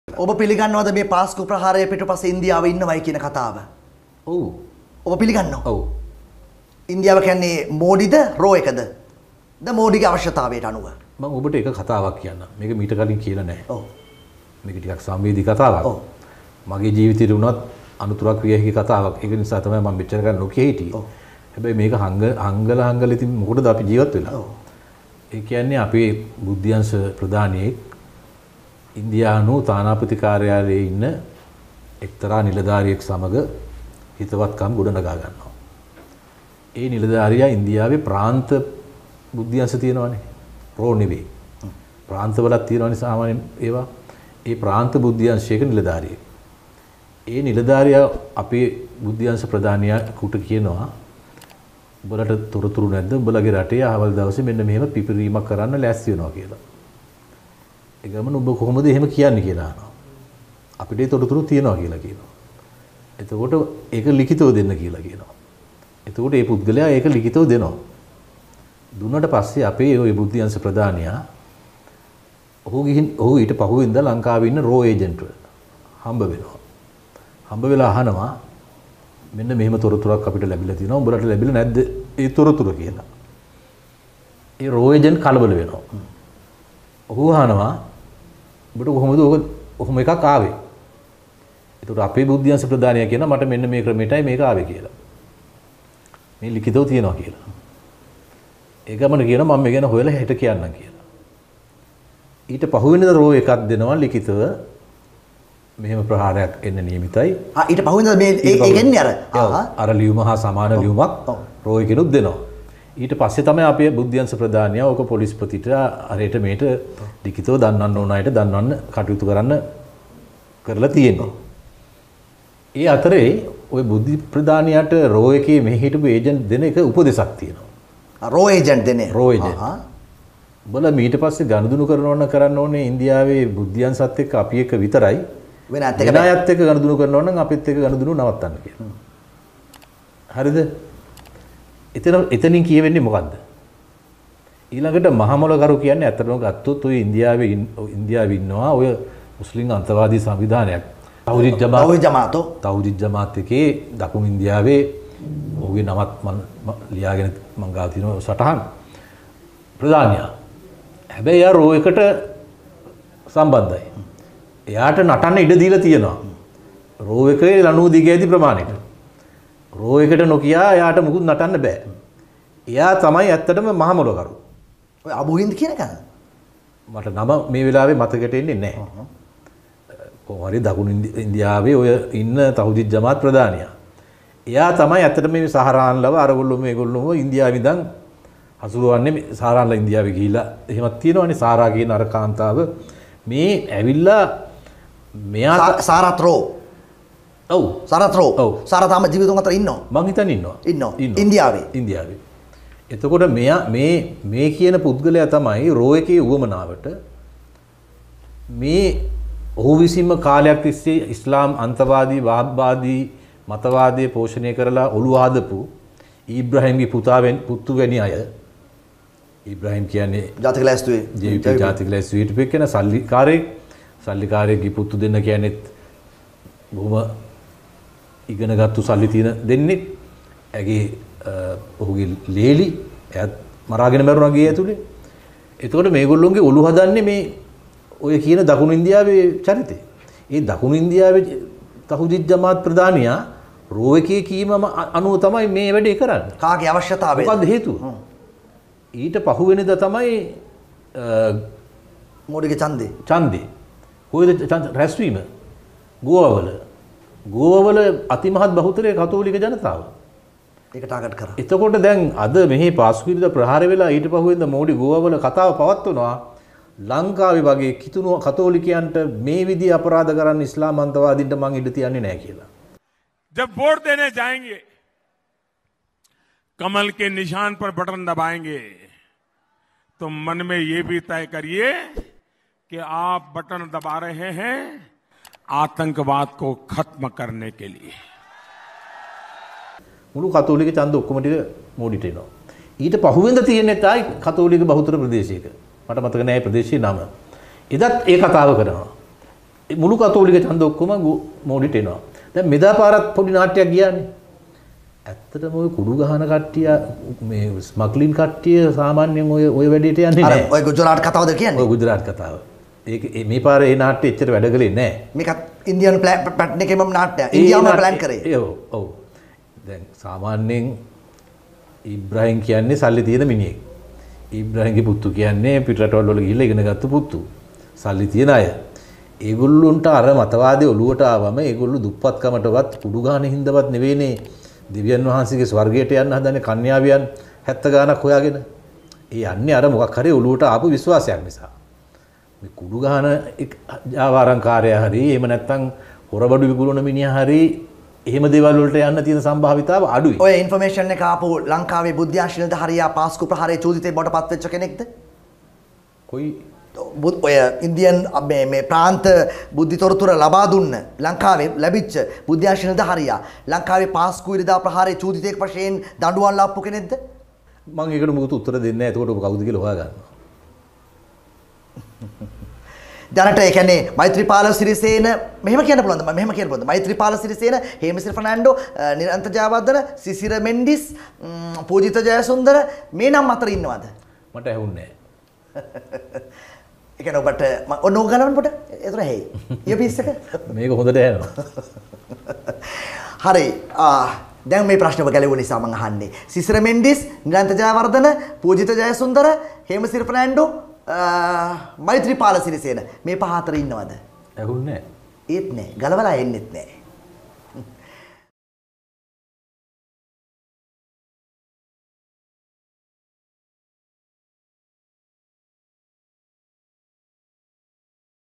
🔴පාස්කු ප්‍රහාරයේ සැබෑ 'මහ මොළකරු' කවුරුන්ද ? ජීවිතය පරදුවට තබමින් විමල් හෙළි කළ රහස !
👇Video credits:
Truth with Chamuditha YouTube Channel